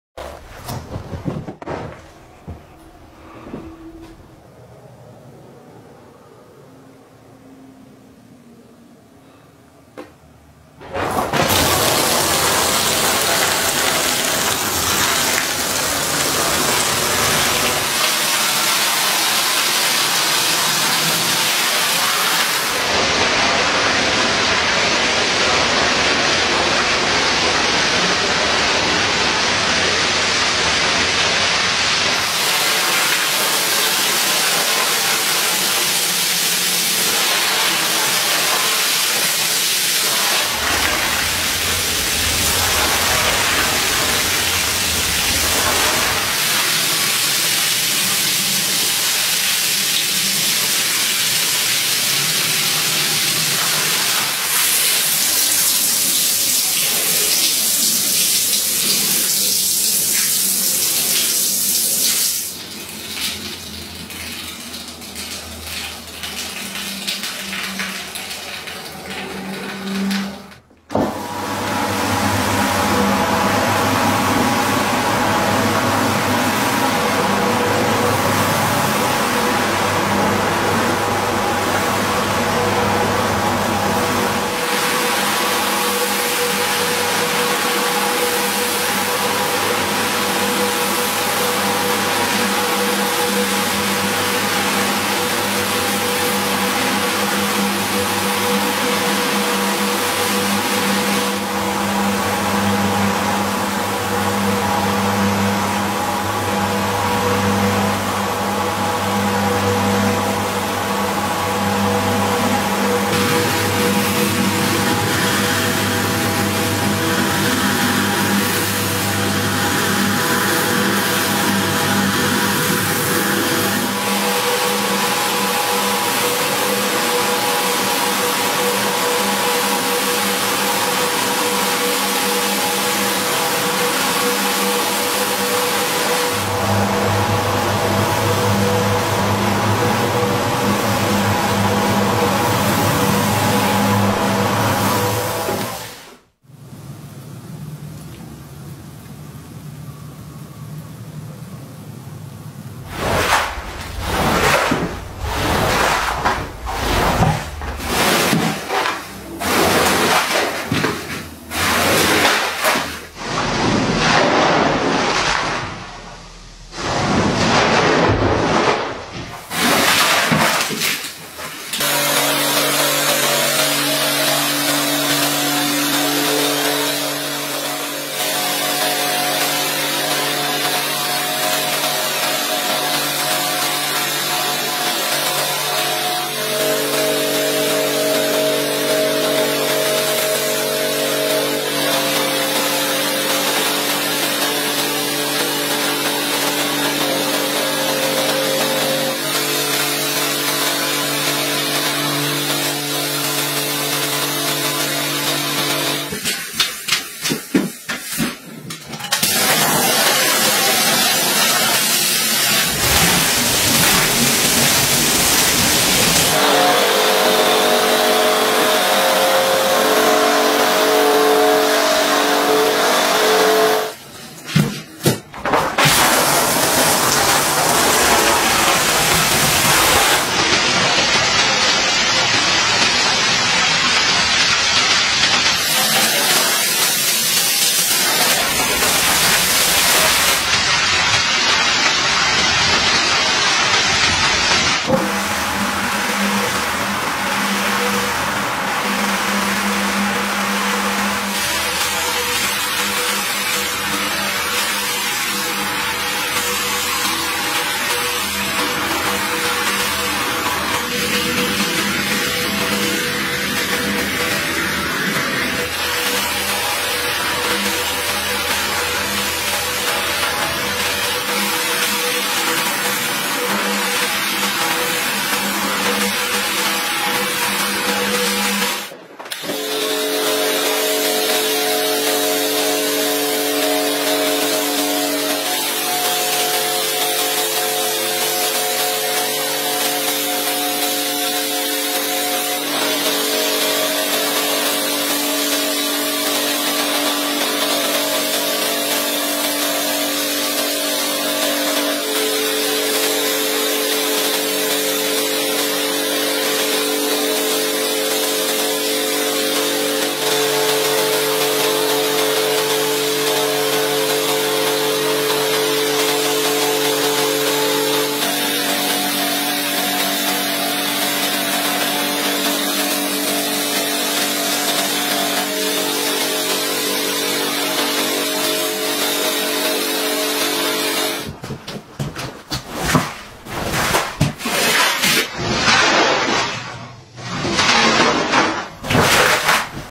Vintage Red Carpet Deep Cleaning
Cleaning a high-quality vintage red carpet that hasn't been washed for many years, are you looking to see if your carpet should be replaced or washed?